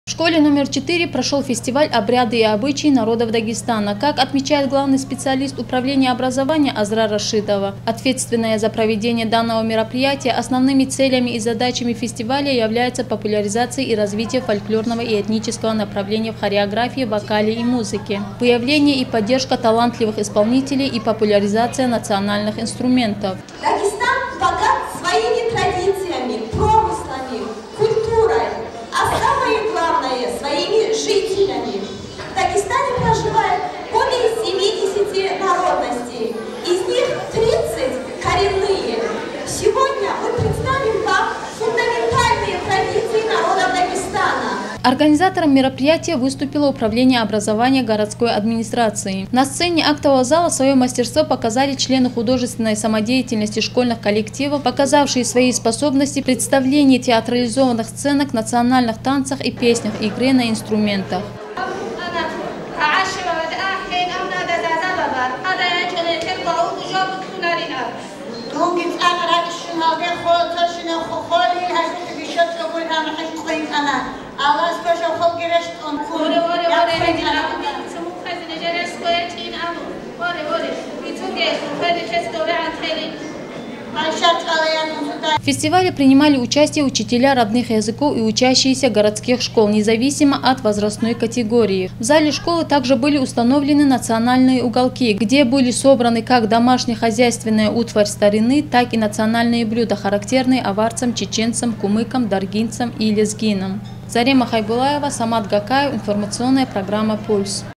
В Хасавюрте провели народный фестиваль в честь года литературы
В городе Хасавюрте прошел муниципальный фестиваль «Обряды и обычаи народов Дагестана», посвященный Году литературы. Организатором выступило Управление образования городской администрации. Все мероприятия фестиваля были проведены в средней школе №4. В качестве участников выступили учителя родных языков и учащиеся школ города, независимо от возрастной категории.